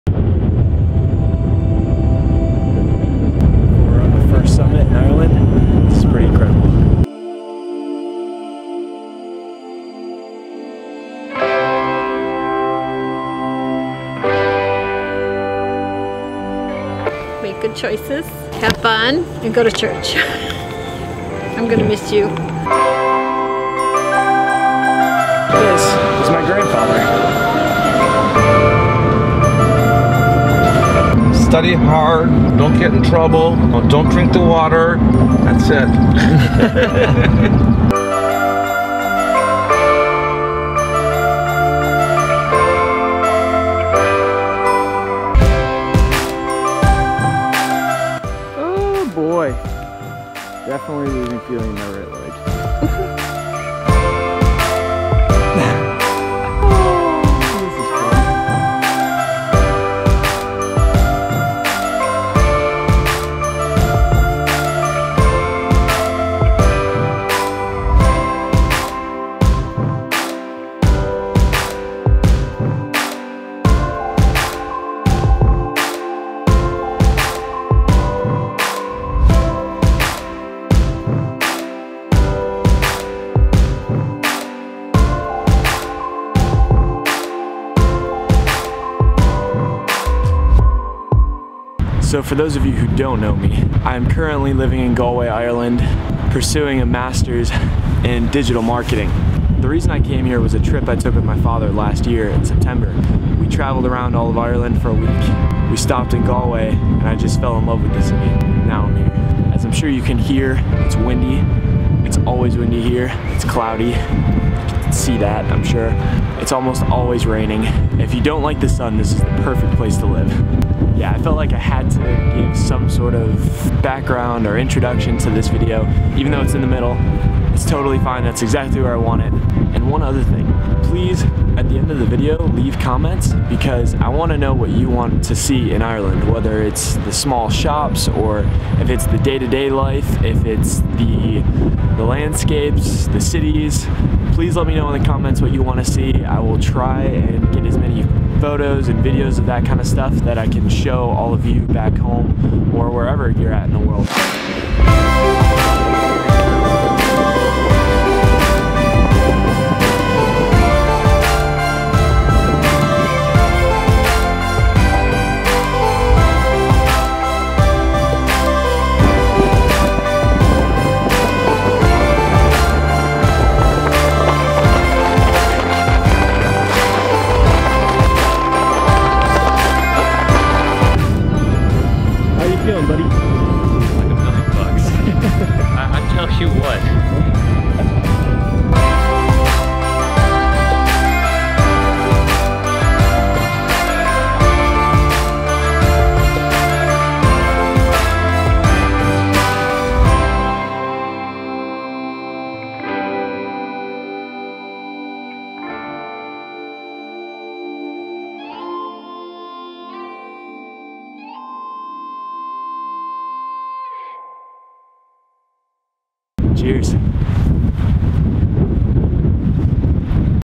Moving to Galway, Ireland
Traveling throughout Western Ireland, including Galway, Connemara National Park, and Leenane. Sharing what it's like to move to a new country and the experiences I've had so far!

VLOG #14 - EPISODE ONE of TheIrelandSeries